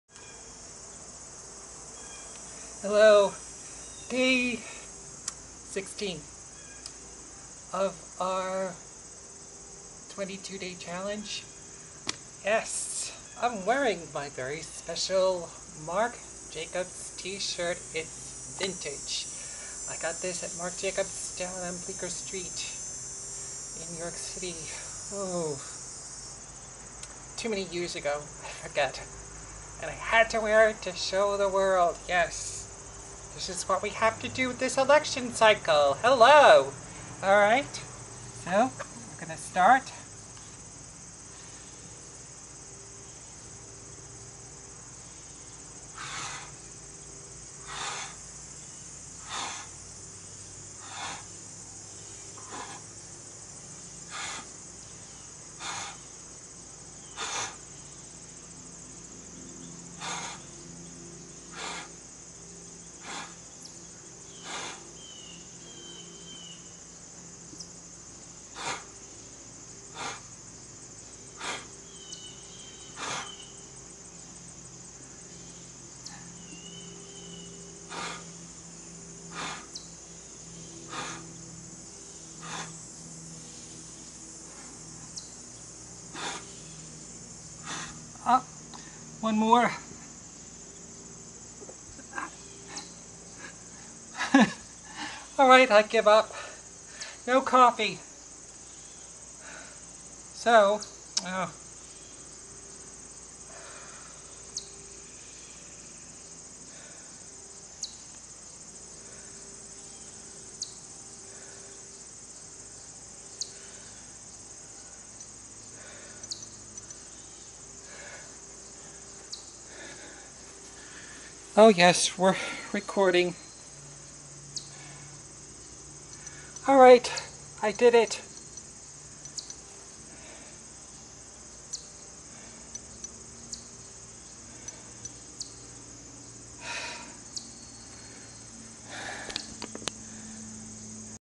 #22 For 22. Day 16. I apologize, technical difficulties ensued.
22Kill doing 22Pushups to show solidarity with the American Armed Forces in their worthy campaign to raise awareness of VETERAN suicide and those that struggle with Post Traumatic Stress Disorder { PTSD }

Are you aware that 22 US veterans commit suicide per day (yes, you read that correctly!)...? Sadly, this situation is echoed in the British former-Service community.

Furthermore, suicide is the single biggest cause of death in men under 45 in the UK.

This is to raise awareness by completing 22 press ups for 22 days followed by a nomination for another person to start the challenge. All serving and retired personnel should when you accept, copy and paste this message to each video.

-Video yourself doing 22 push-ups.
-Post the video to Facebook.
-Feel free to challenge anyone.

The goal is to raise awareness for those who are battling these demons.
Help spread the word and look after each other. Someone you know may be suffering in silence